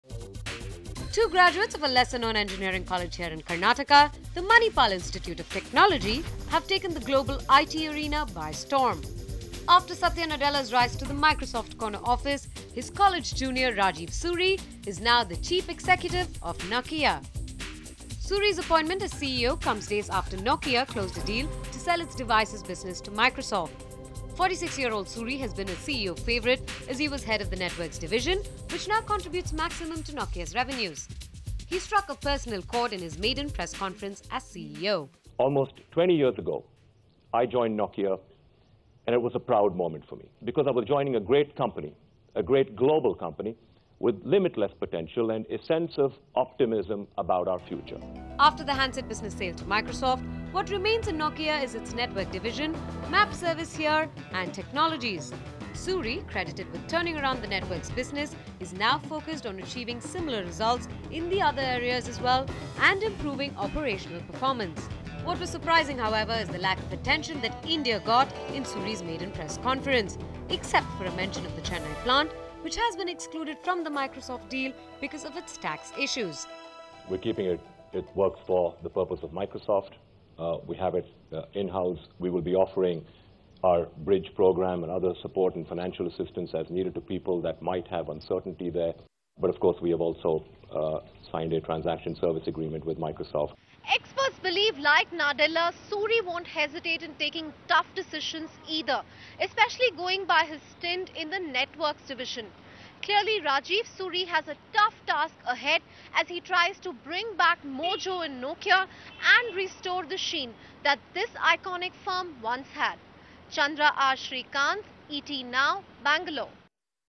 Meet Nokia's NEW CEO: Rajeev Suri
Rajeev Suri has now joined the elite club of Indians -- those who run global firms. Struggling phone maker Nokia named Suri as its CEO, days after it closed a deal to sell its devices business to Microsoft. 46-year old Suri is a Nokia veteran who successfully turned out the company's network equipment division. Can he bring back the sheen in a fading Nokia? ET NOW finds out.